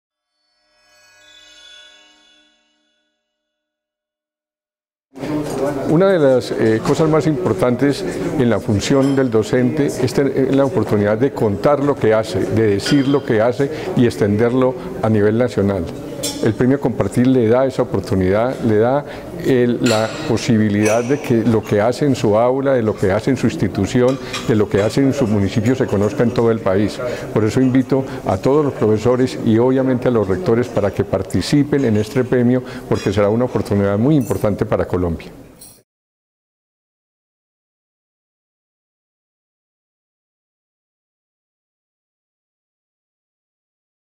Premio Compartir, una oportunidad para reconocer la labor docente
Jaime Bejarano Alzate, Rector de la Universidad La Gran Colombia, seccional Armenia, comparte su opinión sobre el Premio Compartir indicando que es una ventana para visibilizar la labor de los educadores del país y permitir que su gestión sea reconocida.

Lo invitamos a participar en nuestra edición 2016, ingrese a y conozca más detalles.